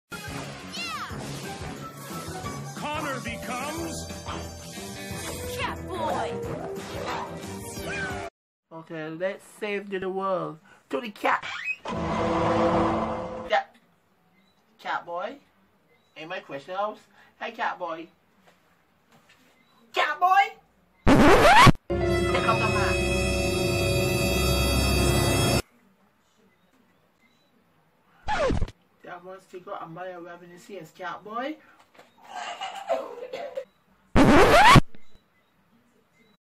When any Parents findout Any Super Hero's Secret Identity
When any Parents findout any Super Hero's secret identity Just like He as Catboy lol 😂😂😂 Don't worry It not gonna happen, the @kylebreitkopf @jacob.ursomarzo @addisonholley PJ Masks is always keep their secret identity at night to save a day. Hope you love my 149th Fan Comedy Videos.